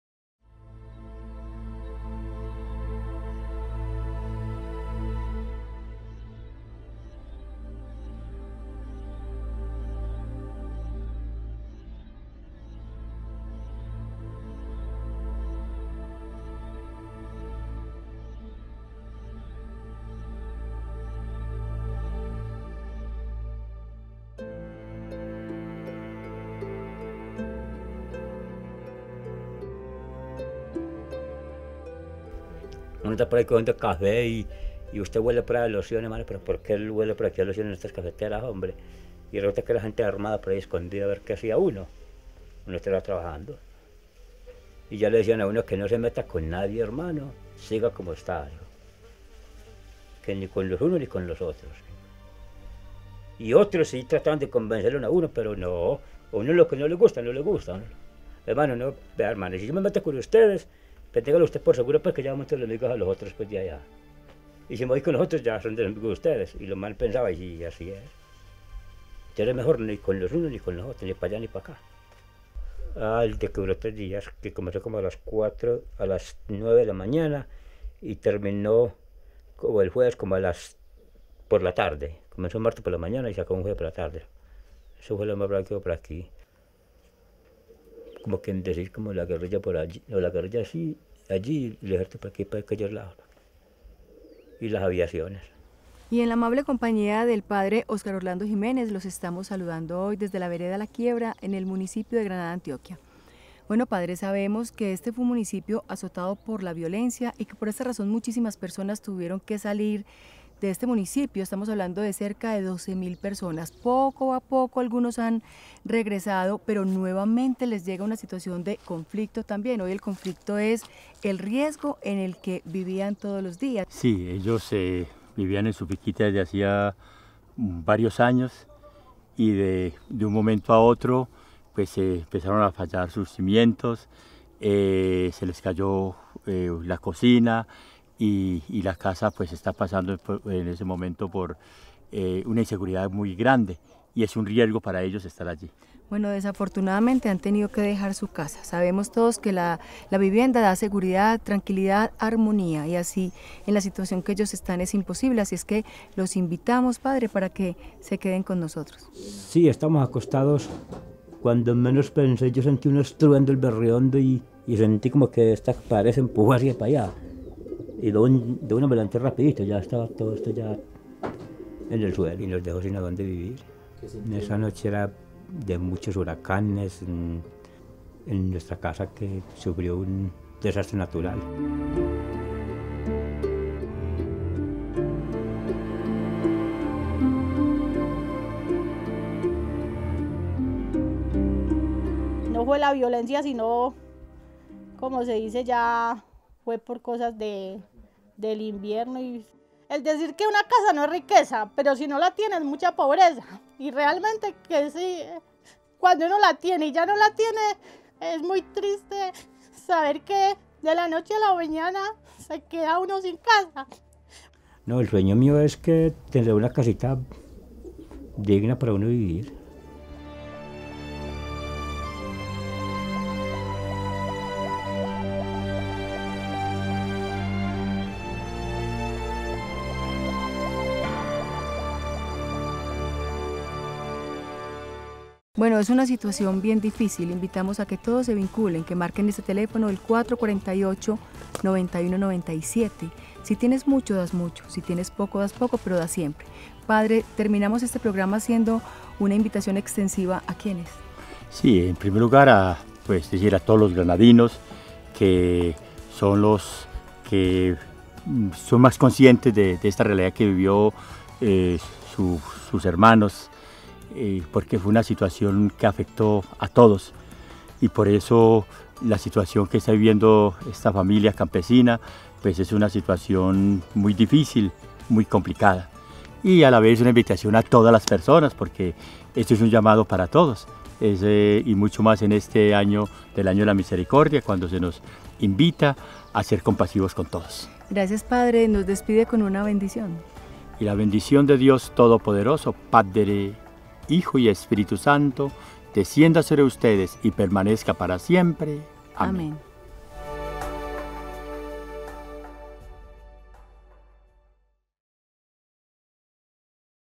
Compasión - Solicitud casa Flia Galeano - Granada - Tele VID
La Fundación Compasión visitó el municipio de Granada en el Oriente antioqueño, lugar en el que conocimos la realidad que viven sus pobladores, quienes vivieron de cerca el fuerte conflicto armado que los azotó por años. La familia Galeano, habitante de la zona, enfrentó su segundo reto, reconstruir los sueños y la fe después de que un alud de tierra destruyera la mitad de su casa, dejándolos en un lugar inseguro para vivir.